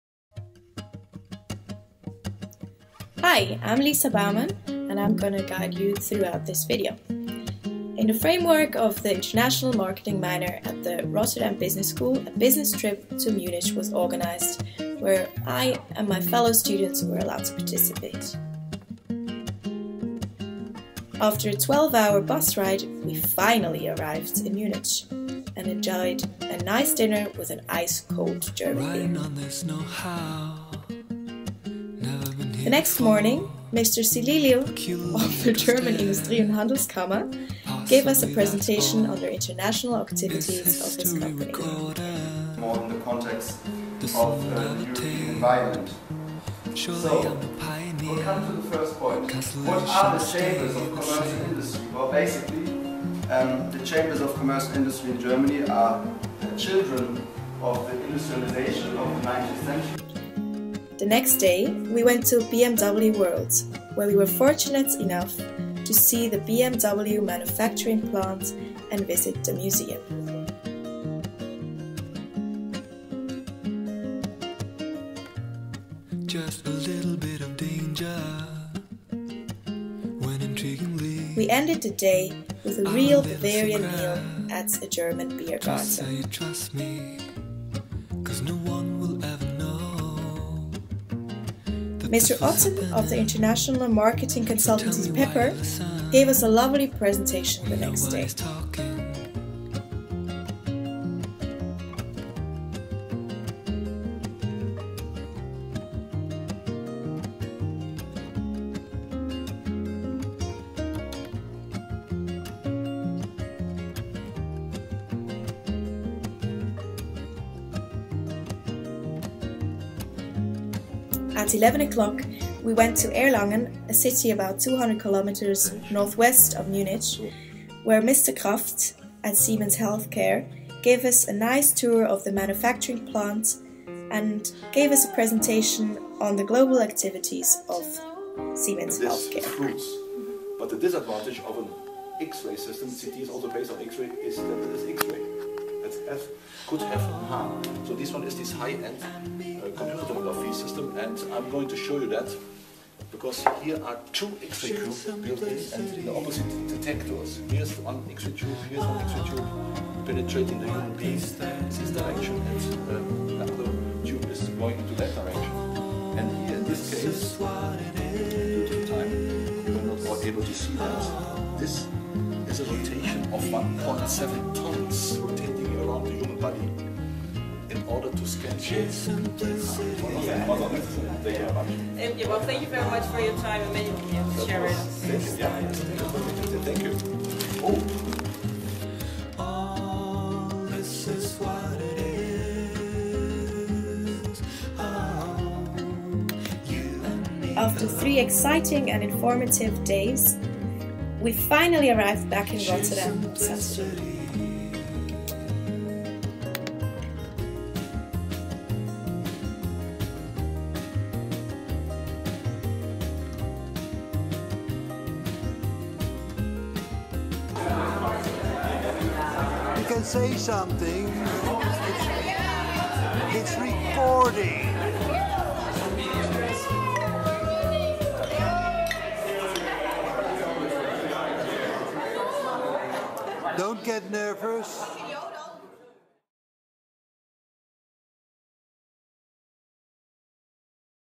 Munich Trip - International Marketing Management Minor
As part of the International Marketing Management Minor at RBS, 20 students experienced an interesting and fun business trip to Munich where they got a glimpse behind the scenes at the marketing departments of 4 large organisations: Chamber of Commerce and Industry (Industrie und Handelskammer), BMW, Pepper Global and Siemens Healthcare diagnostics.